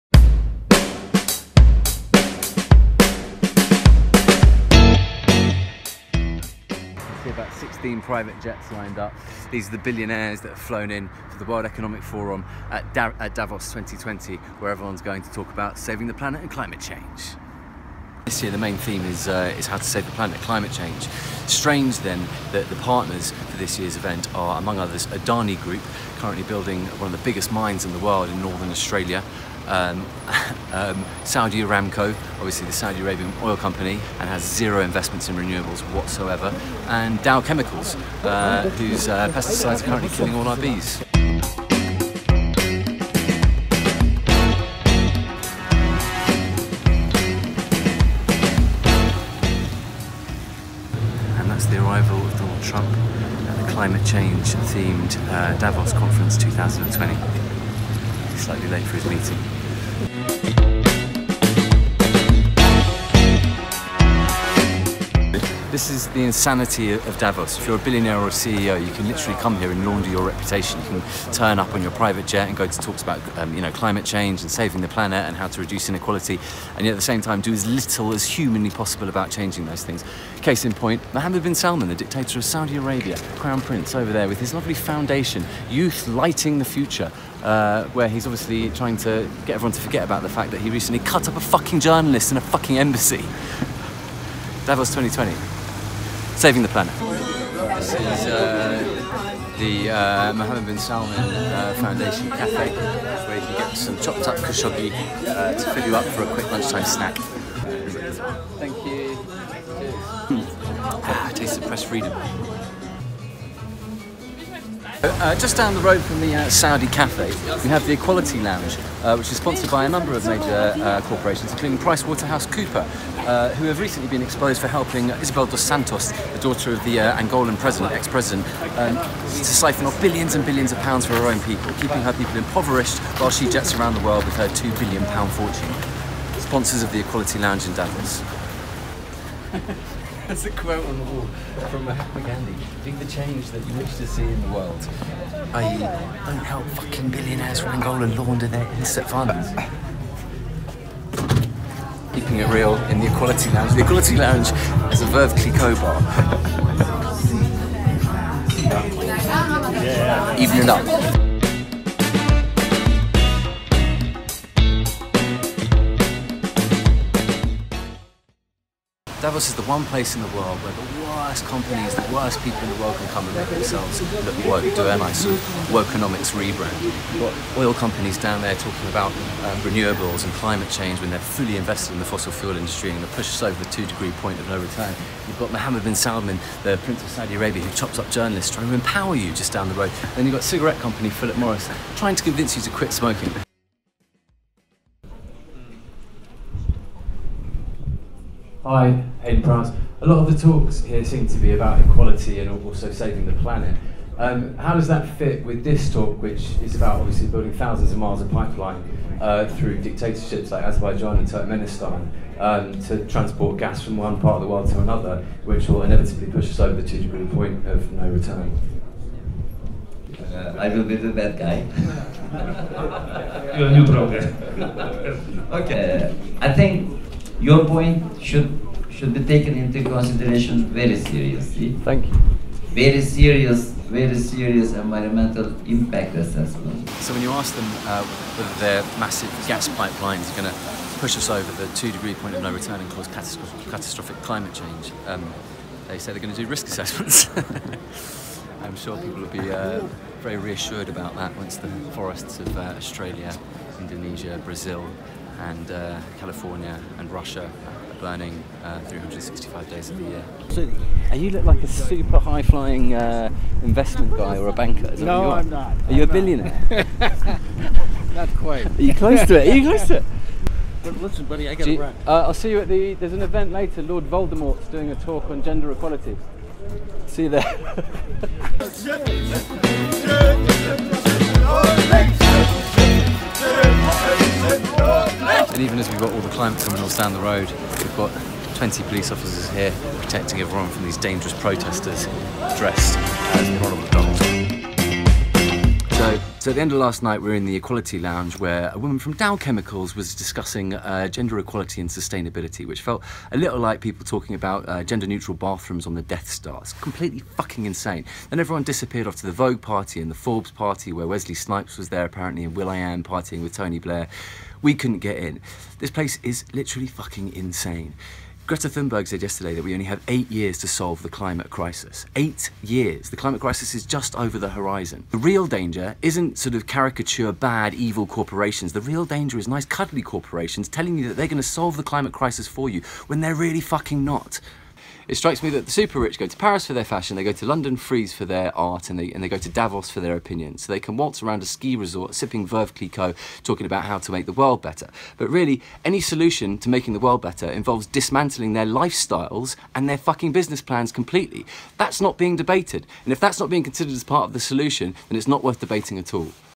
Davos 2020 Wokenomics
The World Economic Forum meeting at Davos this year made 'How to Save the Planet' it's main theme and yet it's official corporate partners are a who's who of climatecriminals. They include Adani Group, which is building a coal mine so big in the world in northern Australia that scientists have warned that it would emit more carbon that a small European country and almost certainly tip the world into climate apocalypse; Saudi Aramco, possibly the most polluting company in the world, which is making zero effort to transition to renewables and which was recently floated on the stock market at a $2tn valuation, funneling some serious cheddar into the pockets of Mohamed Bin Salmen, the Suadi Crown prince who recently assassinated the journalist Jamal Khashogg; and Dow Chemicals, who are sort of Bond-villain-level evil (Google them). The event is wall to wall panel debates and speeches about environmentalism and equality. And yet it's attendees that fly in on private jet are literally a Top Trump deck of corporate bad guys. This capitalist virtue signalling is so dangerous because it is lulling people into a false sense of security that the world's problems are all in hand and soon to be solved by these men in suits. Nothing could be further from the truth. As we all guzzle down their products and services, reassured by their woke PR posturing, the world is in serious danger. We are hurtling towards a 6th mass global extinction. @gretathunburg warned in her Davos speech that we have 8 years to save the planet really Hollywood style ecological catastrophe. If we allow these posturing corporations to mollify our anger we will not change their behaviour enough to prevent this. Get angry. Get out on the streets. Do not let these billionaire parasites sleepwalk us into Armageddon.